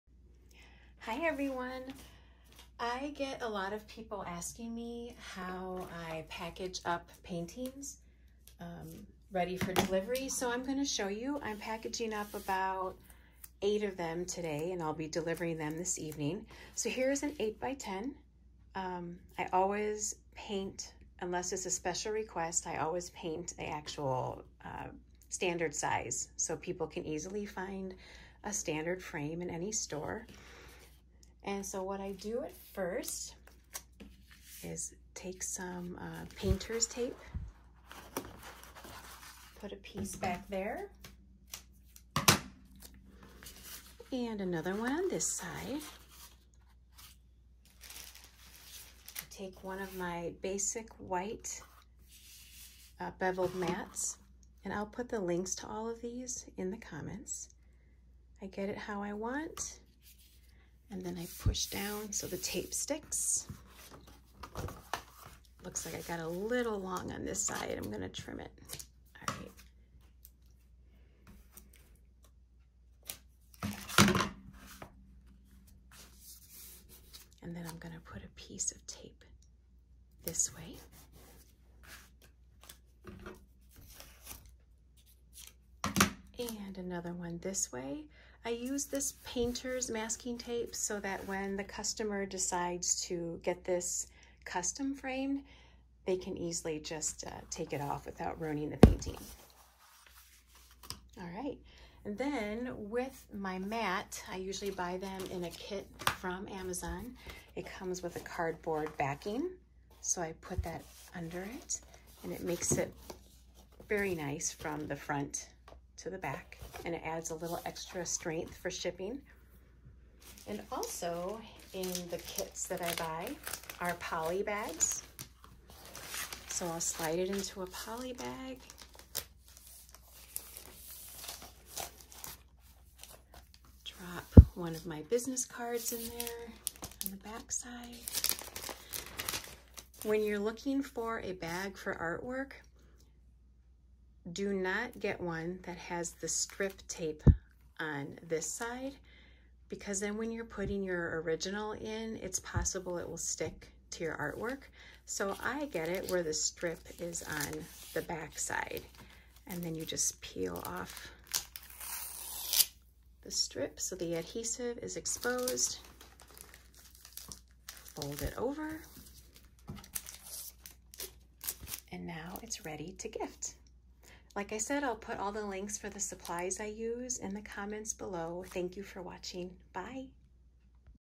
How I prepare my watercolor paintings for delivery
In this short video, I show other artists how to package art so it looks professional and presentable upon delivering to the customer. It costs me only $1.14 for each 8"x10" painting. Here's the link to what I purchase.
Golden State Art, Acid Free, Pack of 50 11x14 White Picture Mats Mattes with White Core Bevel Cut for 8x10 Photo + Backing + Bags